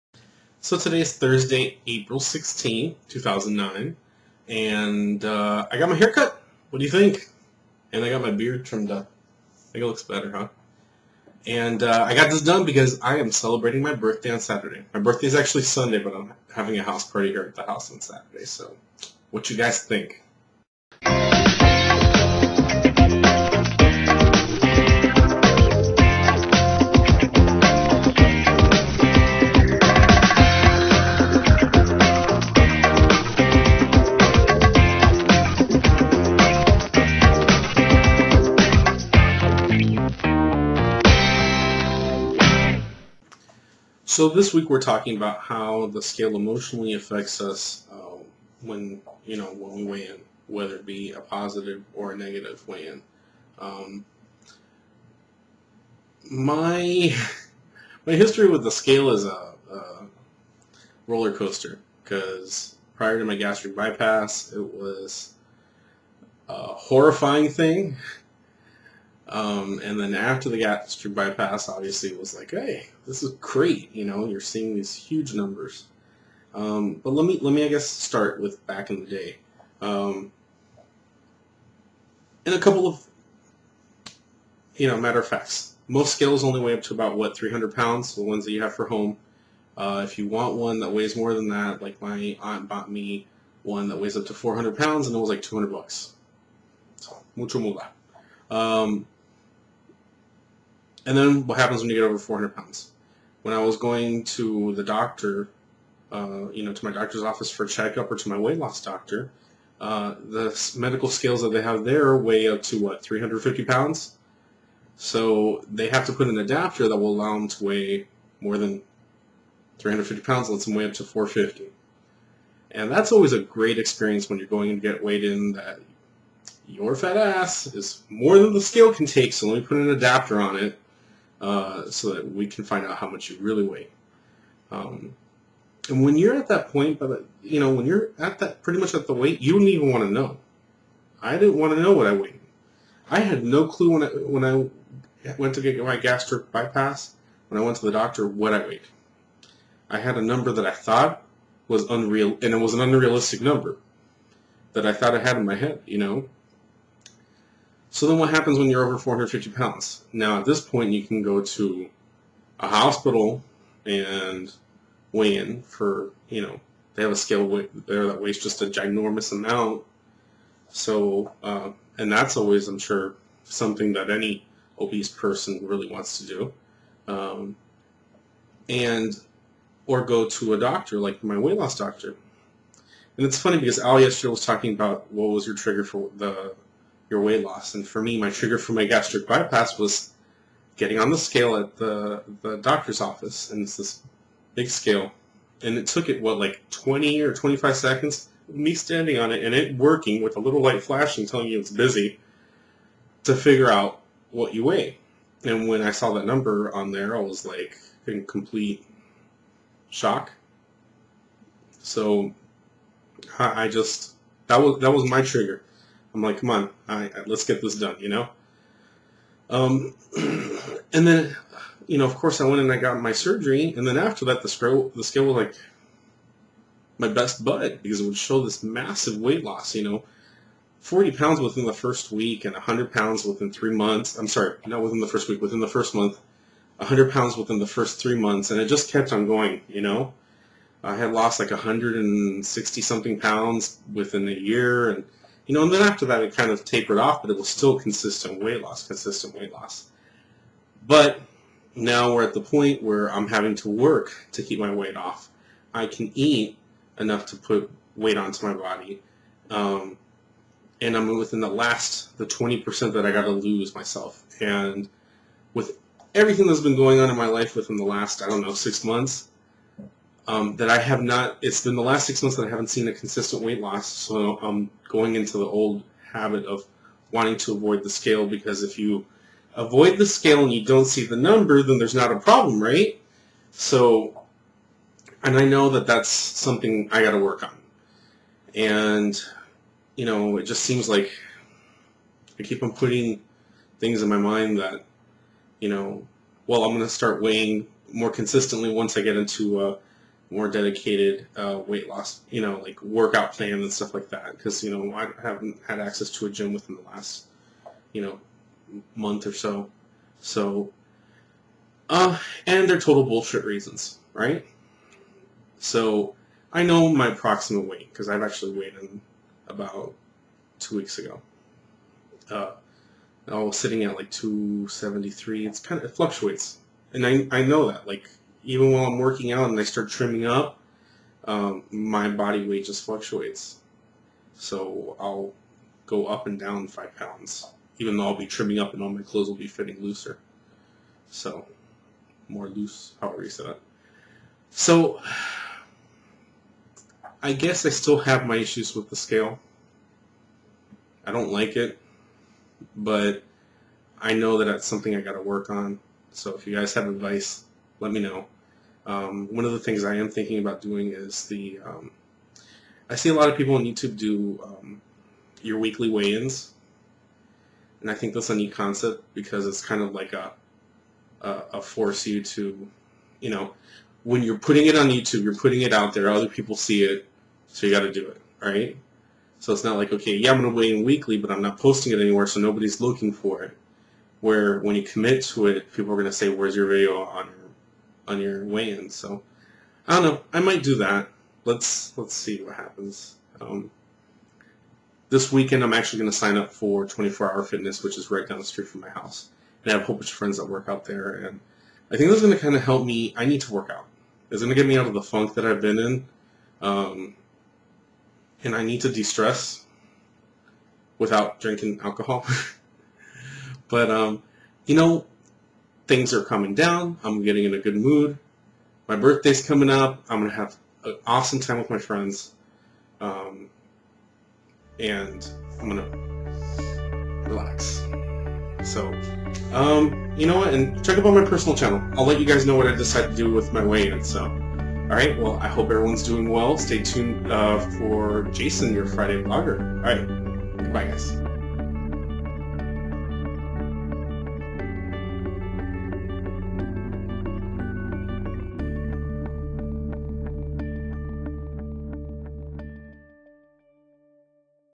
Thurs 04/16 WLL ... Facing the Scale - Avoiding what you don't want to see
Viewer submitted question!
How does the scale affect you emotionally on a gain/loss, and what did/would you do about it in the past or present? [UltimateAnonymity]

How the scale has been a rollercoaster of feelings.

Get involved. Get motivated. Get to GOAL!